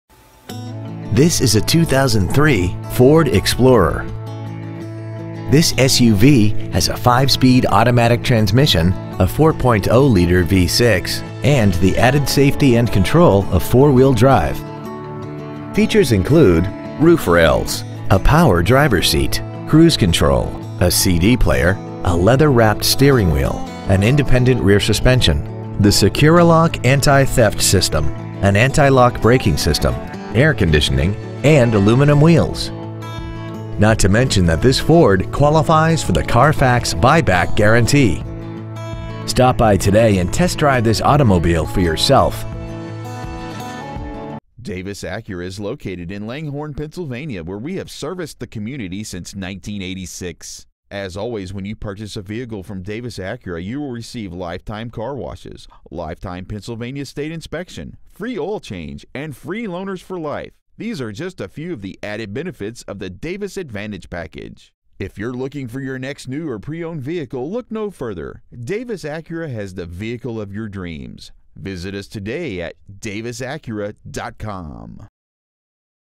Pre-Owned 2003 Ford Explorer Langhorne PA 19047
We have been honored to serve the Langhorne PA area , we promise that your experience at our dealership will exceed your expectations !

 Year : 2003
 Make : Ford
 Model : Explorer
 Engine : Gas/Ethanol V6 4.0L/245
 Trans . : Automatic
 Exterior : Silver Birch Metallic
 Miles : 120,755
 Interior : Graphite
 Stock : 2128061C